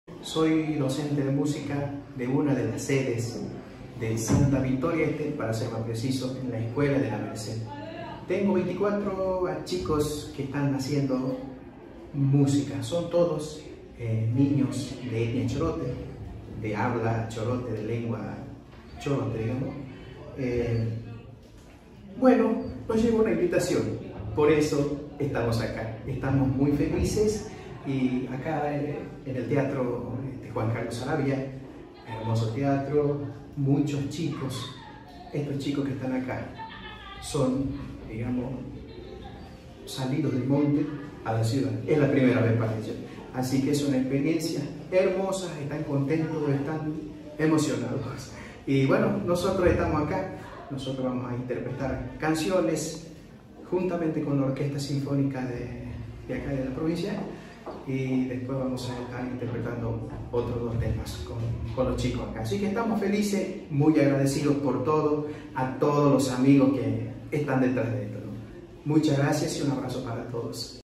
Niños de la comunidad Chorote cantaron junto a la Orquesta Infanto Juvenil en el Teatro Provincial
Nota: Pedro Toledo, profesor de música de la sede Santa Victoria Este del Programa Nacional de Coros y Orquesta.